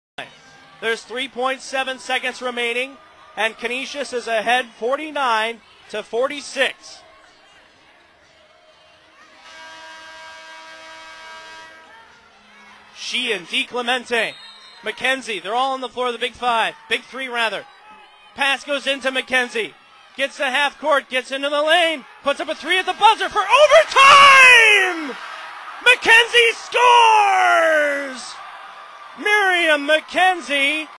Loyola's Miriam McKenzie Hits Buzzer Beater at MAAC Championship
Loyola's Miriam McKenzie hits a 3-pointer at the buzzer to send the 2011 Spark Energy MAAC Women's Basketball Championship quarterfinal against Canisius into overtime.